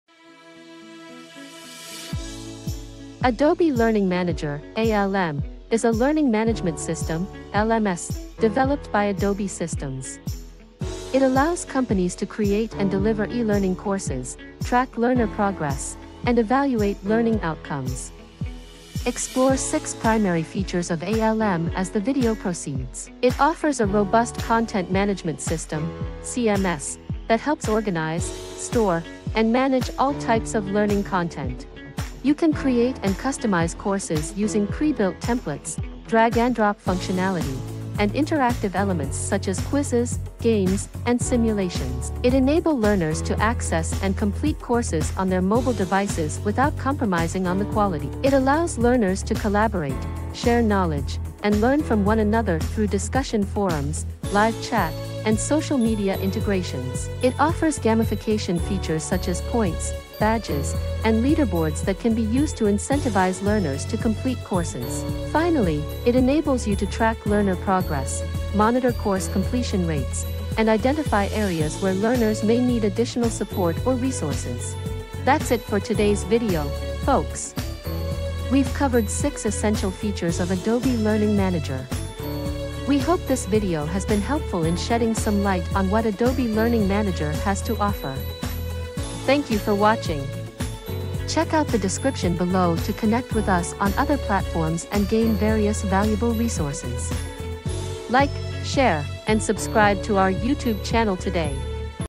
How can you tailor your learning journey with Adobe Learning Manager?
Embark on a journey through Adobe Learning Manager (ALM), a dynamic learning management system by Adobe Systems. Delve into its rich features, from efficient content management to mobile accessibility, collaboration tools, gamification, and meticulous progress tracking. Elevate your learning experience with ALM's versatility and engagement.

To add up to your insights, here are a few use cases of modern-day LMS

1. Compliance Training - Compliance training is an essential aspect of any organization's training program. By offering a centralized platform to deliver and manage compliance training programs, an LMS (Learning Management System) can assist with compliance training. Employers can be assigned training modules, have their progress tracked, and have all required training completed with the help of an LMS, according to training managers.

2. Customer Training - Customer training is a crucial component of every enterprise that provides goods or services. By offering a centralized platform to deliver and administer customer training programs, an LMS (Learning Management System) can assist with customer training. Businesses can build and provide training resources like product tutorials, user manuals, and how-to videos to their consumers using an LMS.

3. Partner Training - By offering a centralized platform to provide and administer training programs for partners, an LMS (Learning Management System) can aid in partner training. Organizations can provide partners with a range of training resources, including product knowledge, sales and marketing training, and technical training, by using an LMS.

Wrapping Up!

Adobe Learning Manager is a potent tool that can help you create engaging and effective learning experiences. Learn more about this cutting-edge learning management system by tuning in. Additionally, you can view this webinar recording if you're still unclear about the need for a second LMS for your partners and clients.